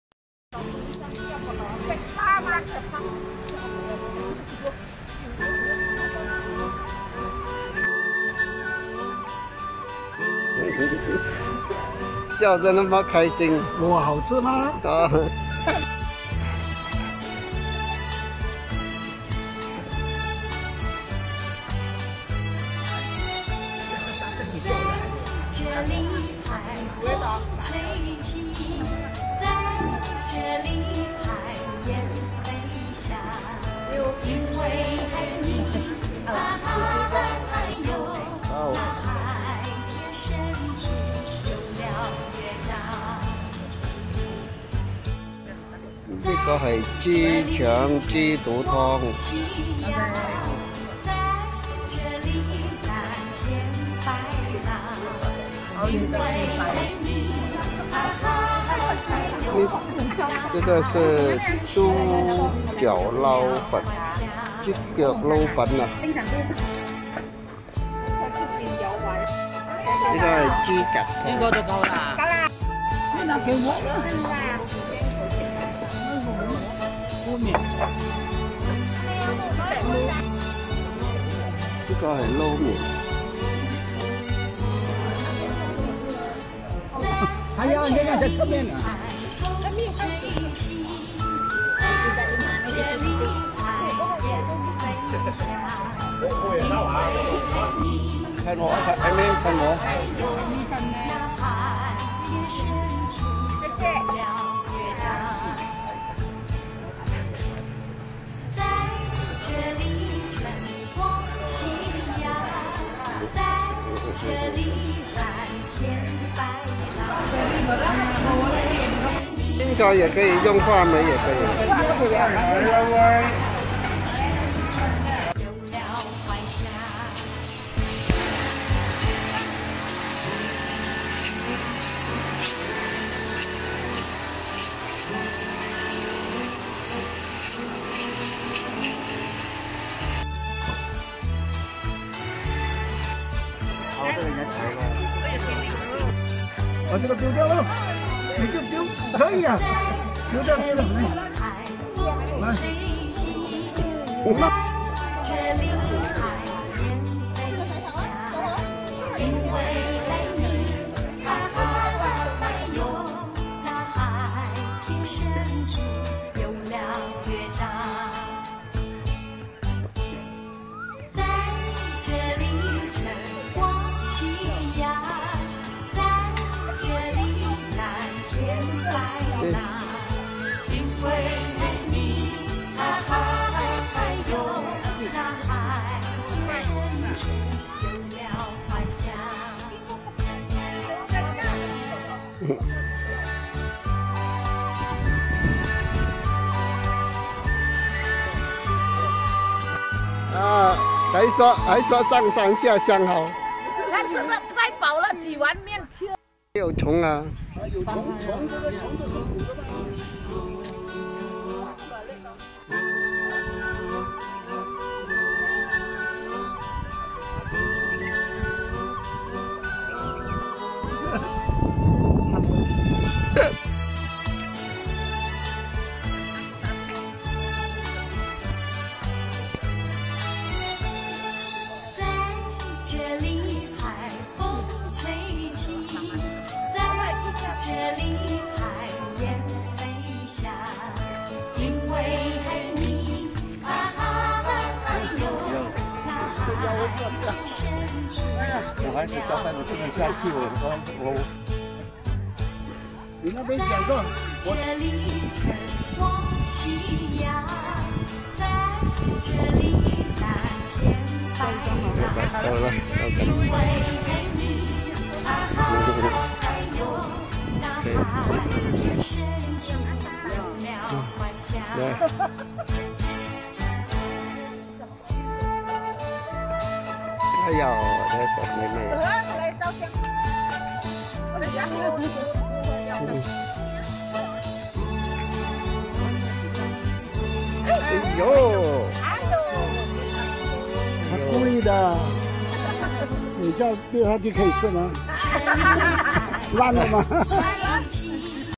尋找客家腌面的故事
尋找好食客家腌面来到元朗，再到大棠生态园宋摘士多啤梨。